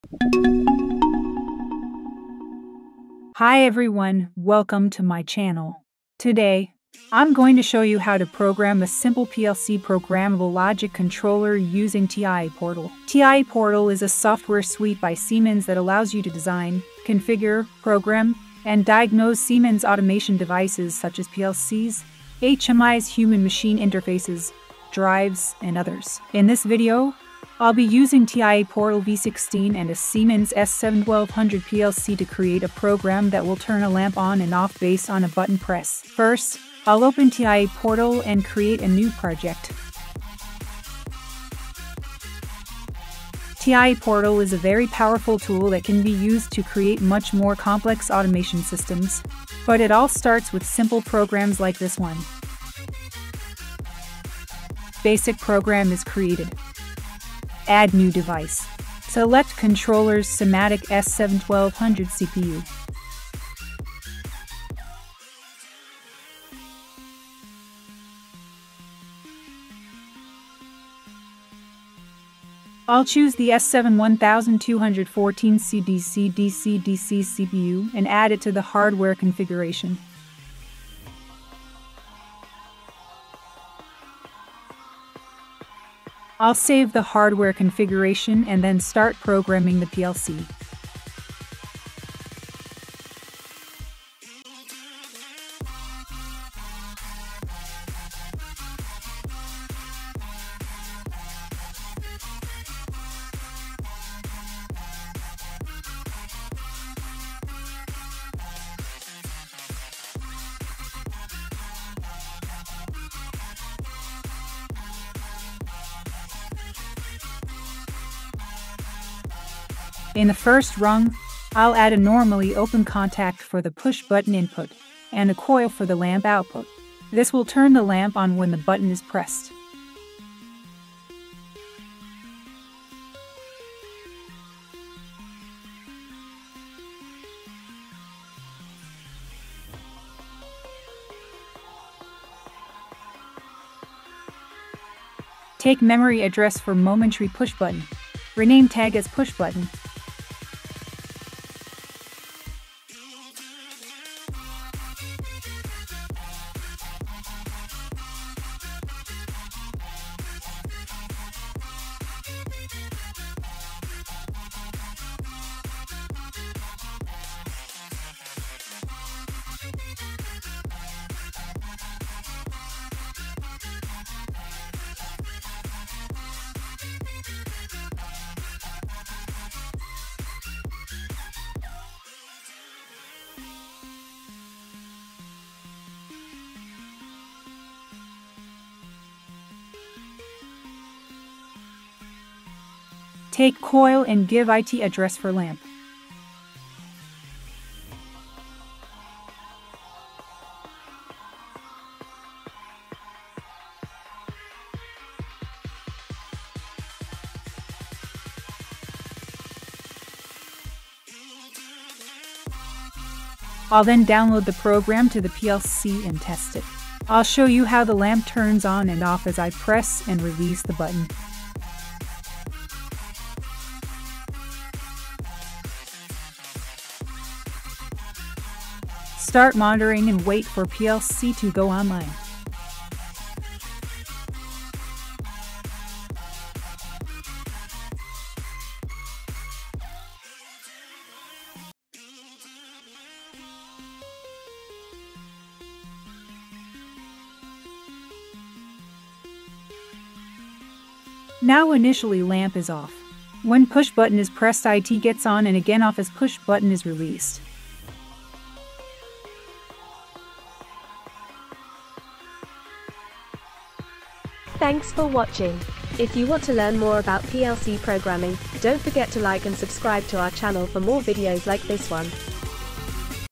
"MY FIRST PLC PROGRAM !!! "| "My First PLC Program: Step-by-Step Tutorial"
Today, I'm going to show you how to program a simple PLC (programmable logic controller) using TIA portal.
TIA portal is a software suite by Siemens that allows you to design, configure, program, and diagnose Siemens automation devices such as PLCs, HMIs (human-machine interfaces), drives, and others.
In this video, I'll be using TIA portal V16 and a Siemens S7-1200 PLC to create a program that will turn a lamp on and off based on a button press.

First, I'll open TIA portal and create a new project. I'll choose the S7-1214C DC DC DC CPU and add it to the hardware configuration.
I'll save the hardware configuration and then start programming the PLC.

In TIA portal, programming is done using the ladder diagram language. I'll create a new block for my program and add rungs to it.
This will turn the lamp on when the button is pressed.

I'll then download the program to the PLC and test it. I'll show you how the lamp turns on and off as I press and release the button.
Conclusion:

And that's it! In just a few minutes, we were able to create a simple PLC program using TIA portal.
TIA portal is a very powerful tool that can be used to create much more complex automation systems, but it all starts with simple programs like this one.